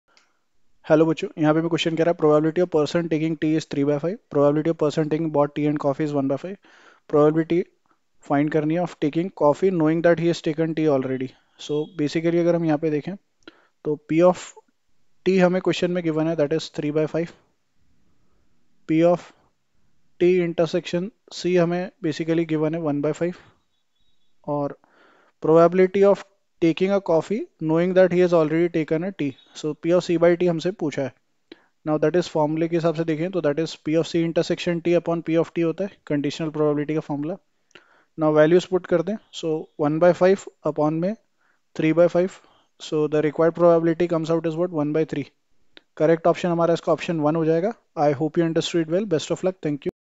The probability of a person taking tea is 35&nbsp;and the probability of a person taking both te....
The probability of a person taking tea is 35&nbsp;and the probability of a person taking both tea &nbsp;coffee is 15,, then find the probability of taking coffee knowing that he has taken tea already?